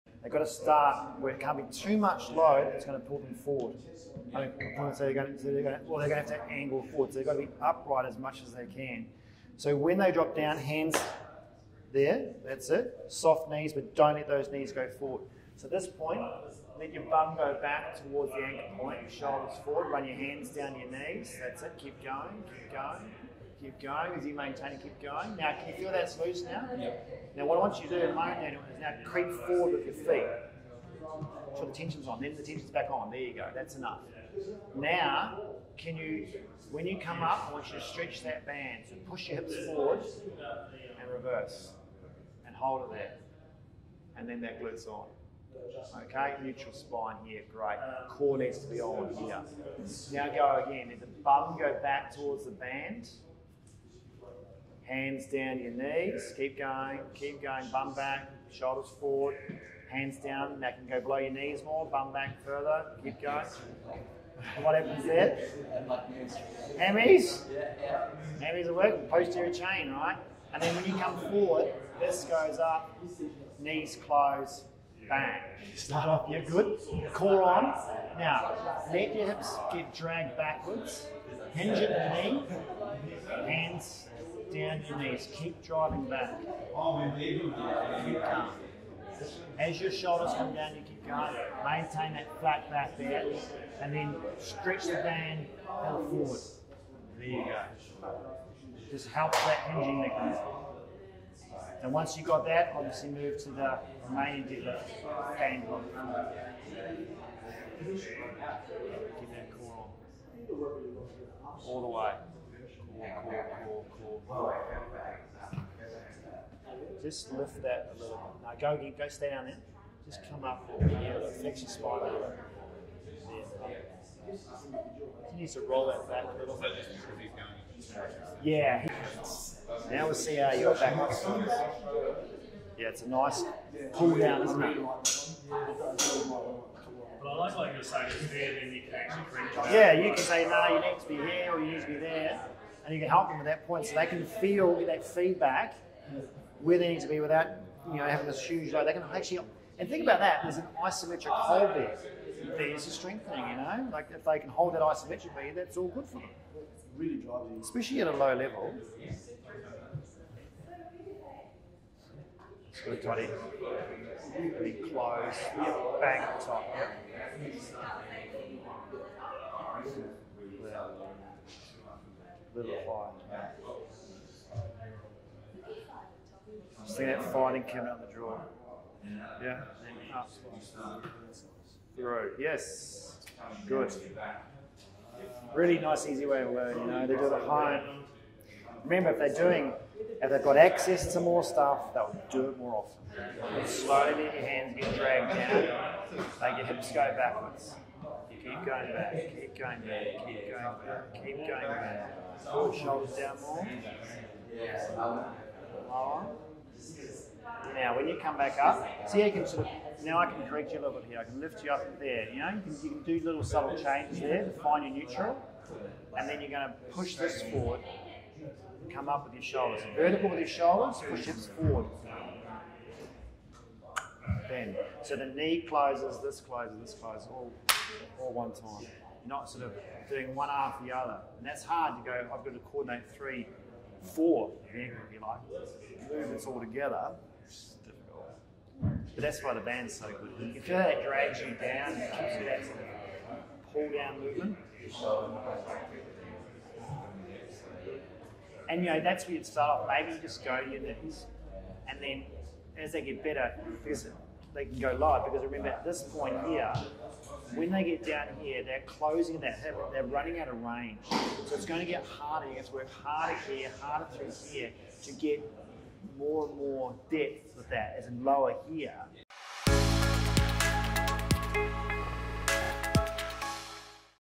Course Workshop: Hip hinge and Deadlift using Bands | Tim Keeley | Physio REHAB
Insight into the workshop modules of the lumbar spine course (face-2-face and online). Here I am helping and instructing other Physio's to feel the correct technique and action of the hip hinge and the deadlift using a powerband for resistance and feedback - to perfect the movement. Practitioners have to be able to perform the exercises well themselves before then can teach patients effectively.